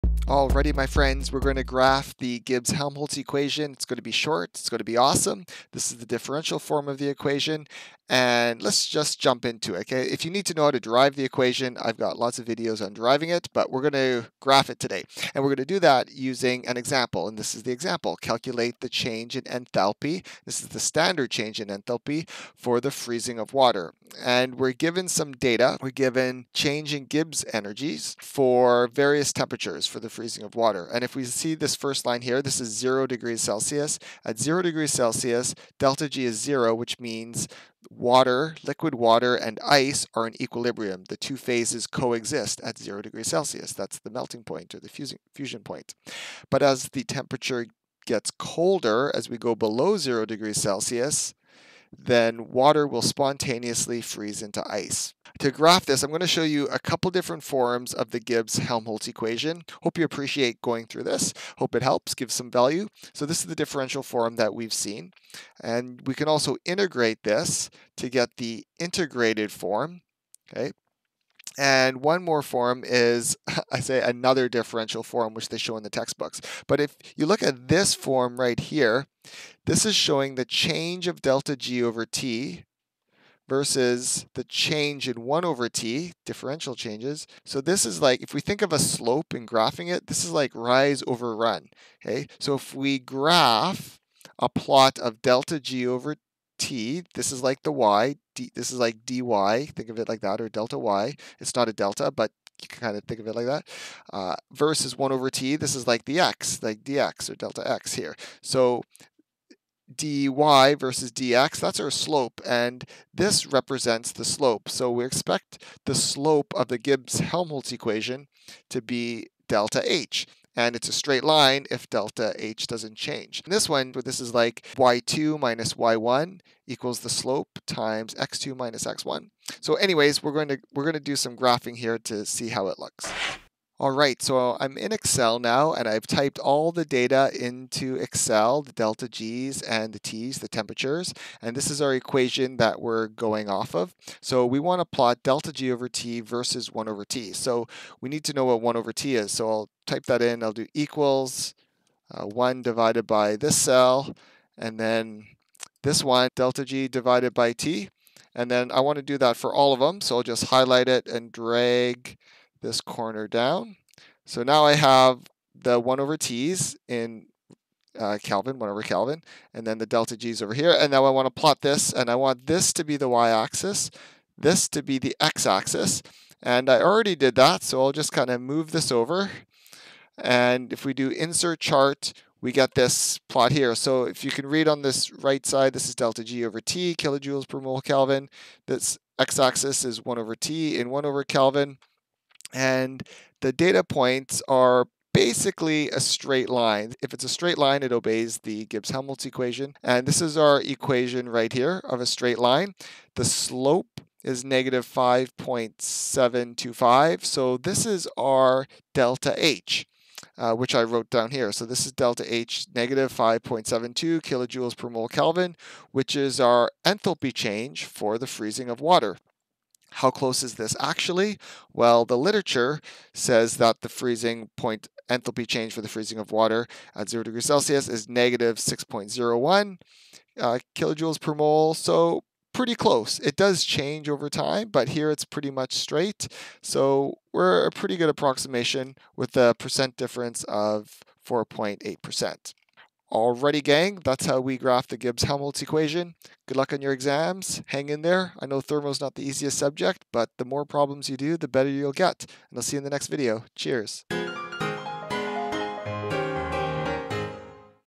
Graph the Gibbs-Helmholtz Equation 💪
Graph the Gibbs-Helmholtz equation using data! The Gibbs-Helmholtz equation is important in thermodynamics as it shows how the change in Gibbs energy is dependent on temperature.